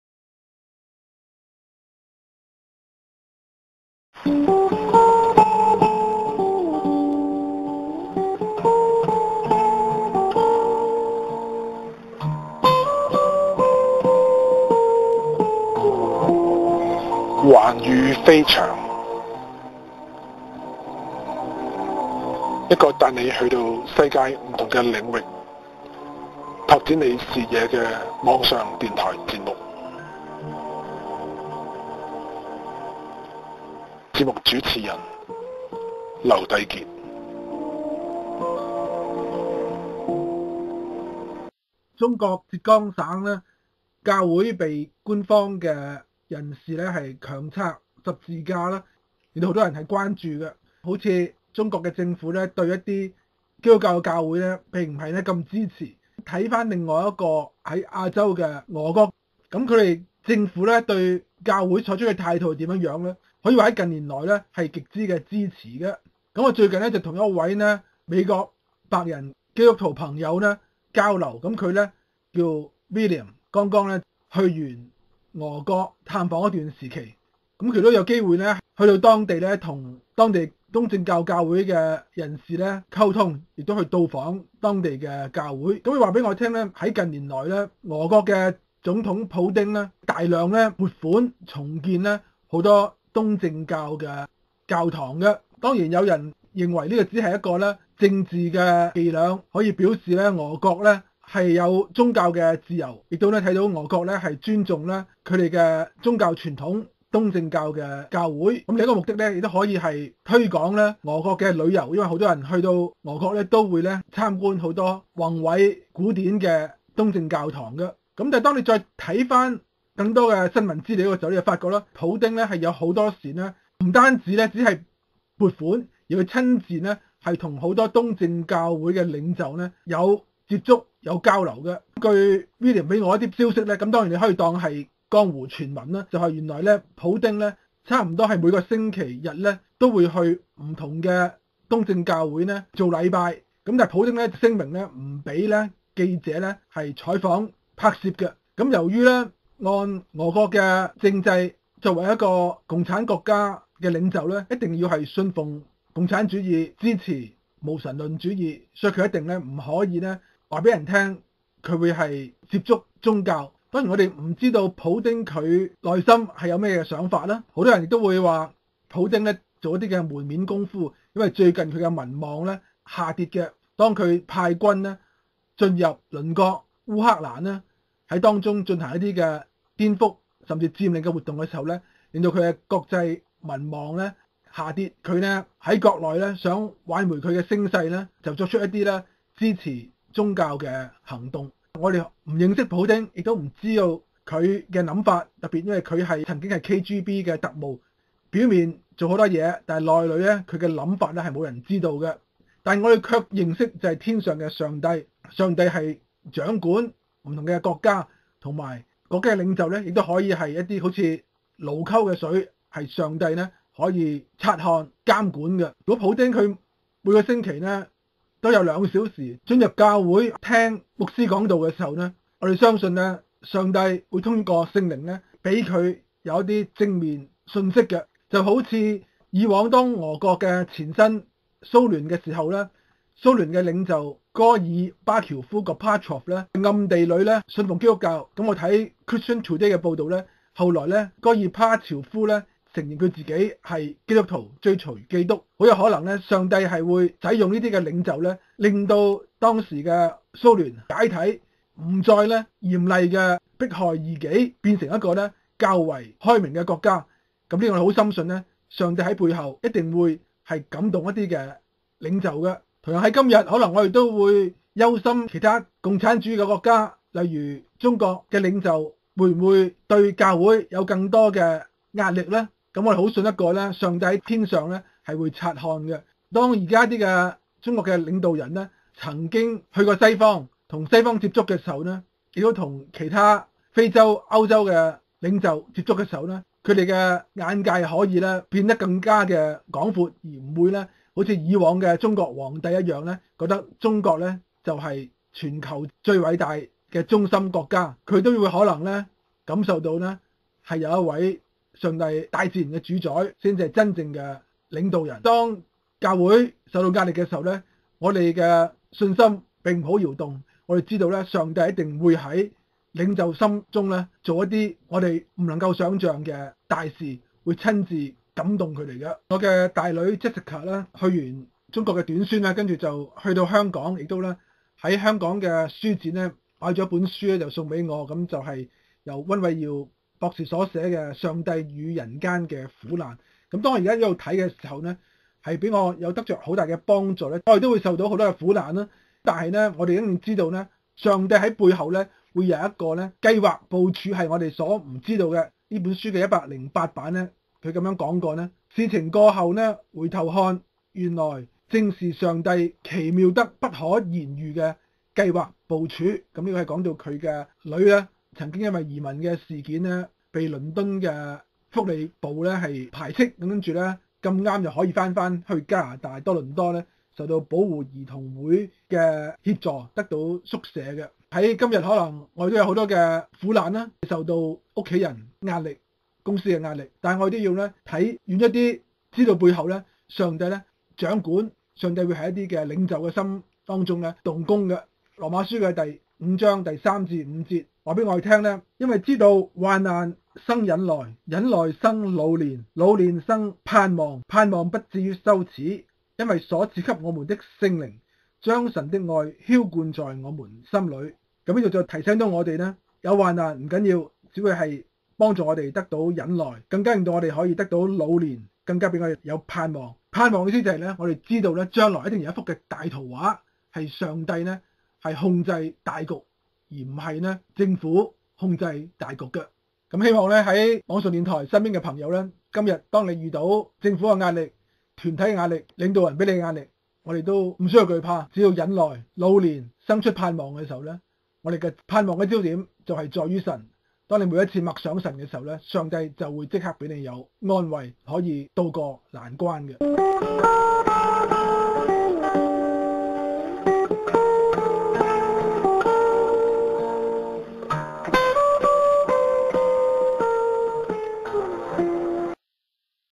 第287集：俄國教會得政府領袖支持，是利是弊？
當共產主義國家中國在浙江一些教會被地方權力人士拆下十字架時，前共產主義國家俄國的教會情況又如何？

筆者與美國朋友交流，他剛探訪俄國教會，得悉俄國政府近年頗支持教會，不是清拆，而是重建。據當地江湖傳聞，俄國領袖普丁更差不多每主日都參加教會祟拜。但所為何因？俄國教會得政府領袖支持，教會內政會受干擾？基督福音會被廣傳？究竟是利是弊？